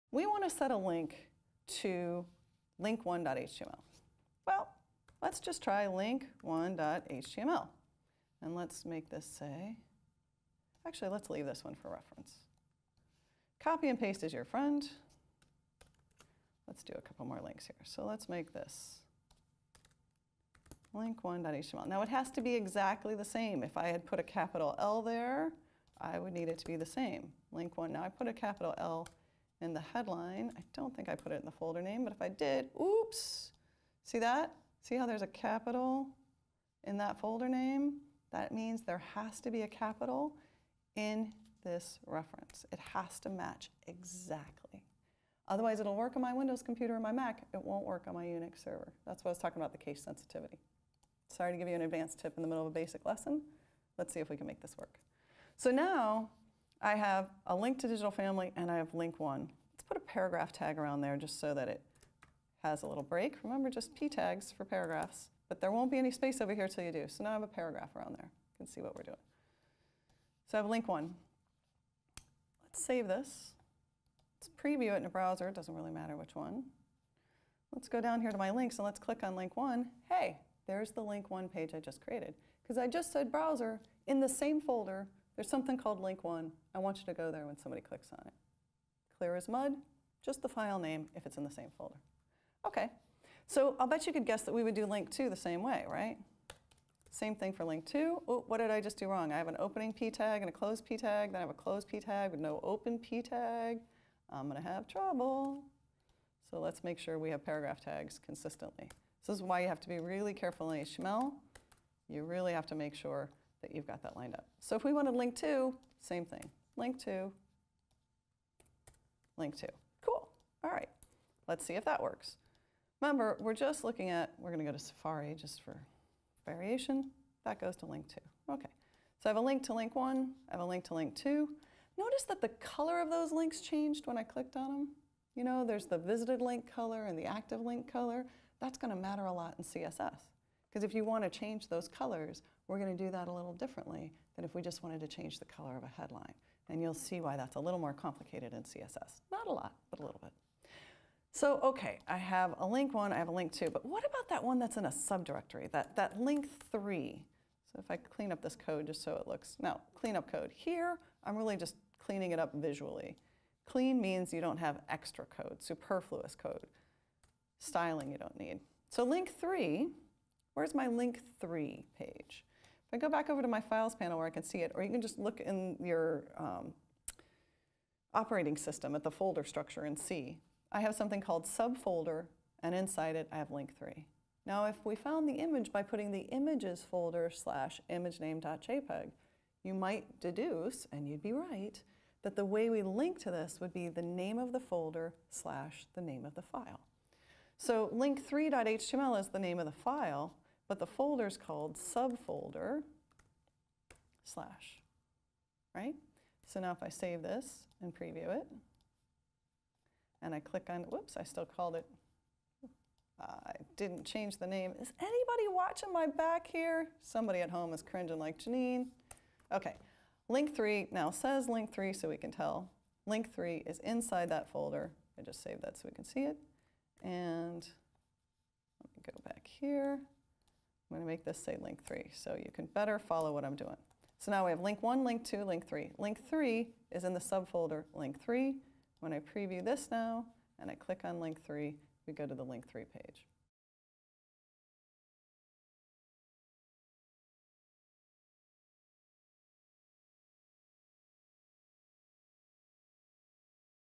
Setting a HTML Link with Janine Warner
In this clip from the creativeLIVE course, Intro to HTML and CSS: Beyond Templates, Janine Warner demonstrates the process of setting up a HTML link in a style sheet.

In her course, Warner teaches web designers how to customize web sites using basic HTML and CSS.